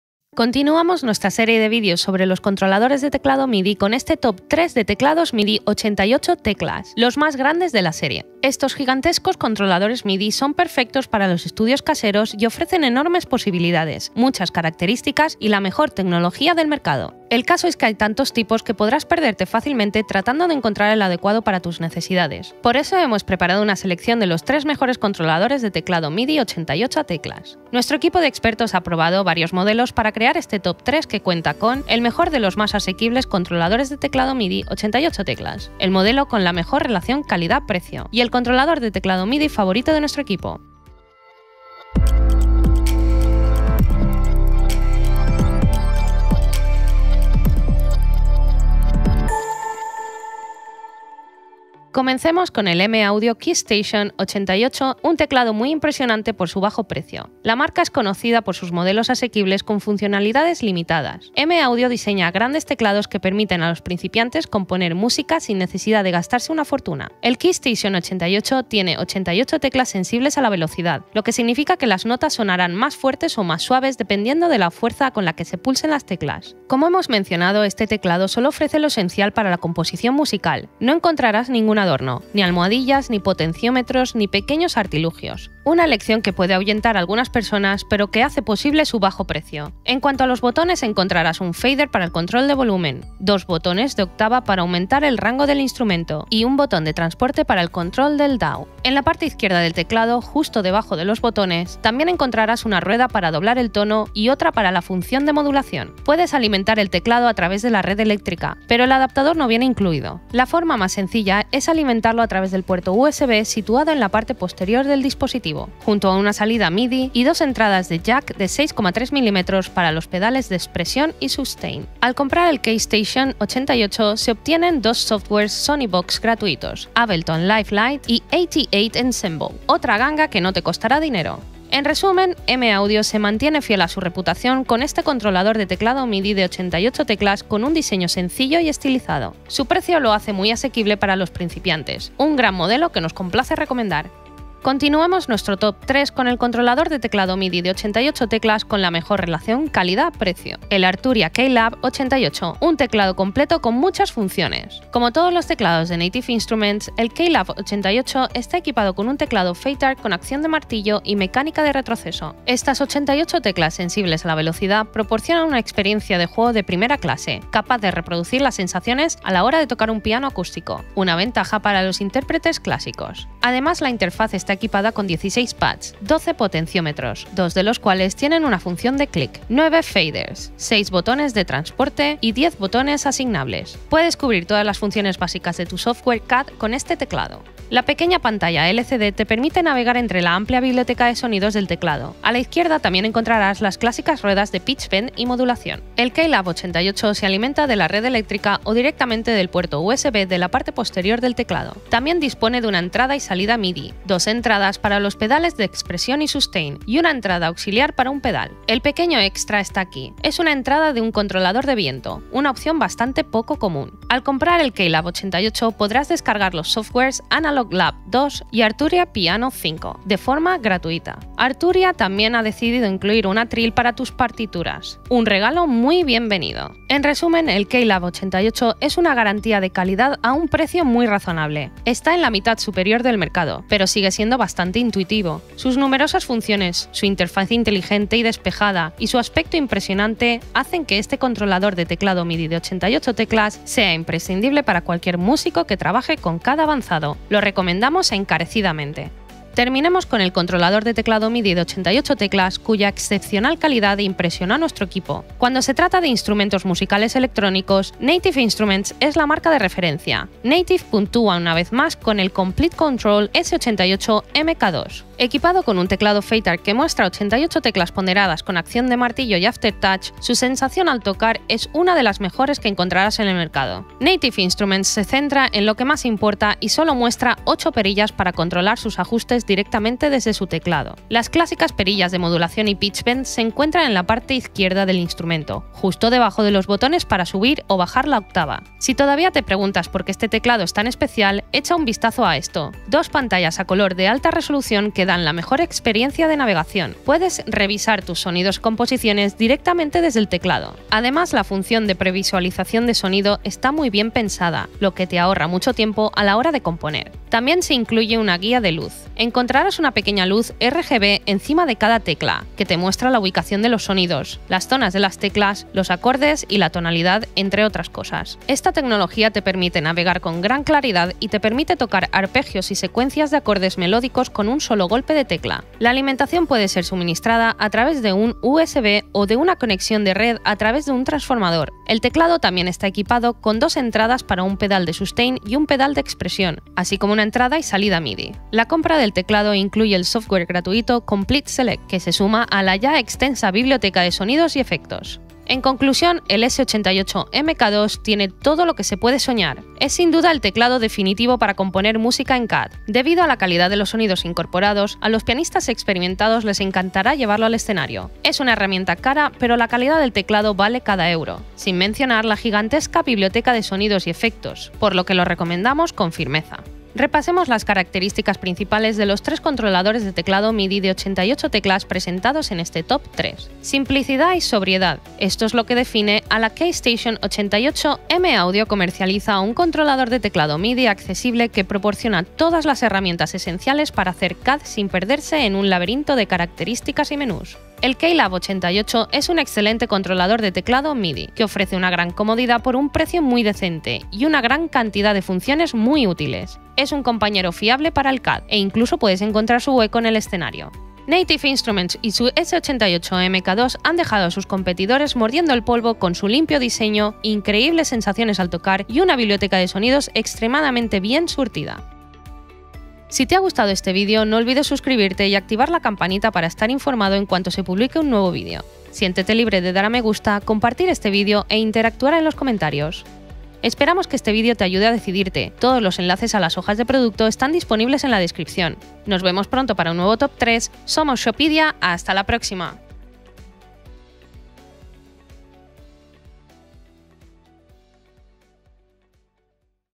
🏆 TOP 3 MEJORES TECLADOS MIDI 88 TECLAS 2023 ( COMPARACIÓN Y ANÁLISIS )
⚠️ Toda la información sobre los 3 mejores Teclados MIDI 88 Teclas está disponible en los siguientes enlaces :

00:00 Introducción
00:51 M-Audio Keystation 88 MK3 Análisis
02:24 Arturia KeyLab 88 MKII Análisis
04:08 Native Instrusments Komplete Kontrol S88 MK2 Análisis
06:07 Comparación de Teclados MIDI 88 Teclas
06:51 Outro

➤ Continuiamo la nostra serie di video sulle tastiere midi con questa Top 3 sulle 88 tasti, le più grandi della serie. Questi gigantesche tastiere MIDI sono perfette per gli home studio e offrono enormi possibilità, molte funzionalità e la migliore tecnologia presente sul mercato.

➤ Le tastiere midi a 88 tasti ti consentono di suonare strumenti virtuali sul tuo computer. I loro 88 tasti rendono qualsiasi strumento impressionante. Tuttavia, sono perfetti per un uso prolungato in un home studio.

➤ Il fatto è che ce ne sono così tanti che potresti facilmente perderti cercando di trovare quella giusta per le tue esigenze. Ecco perché abbiamo preparato una selezione delle 3 migliori tastiere midi a 88 tasti.

➤ Il nostro team di esperti ha testato diversi modelli per creare questa top 3 che presenta:
• la miglior tastiera midi a 88 tasti tra le più economiche,
• la miglior tastiera midi a 88 tasti al miglior rapporto qualità/prezzo
• e la tastiera midi a 88 tasti preferita dal nostro team.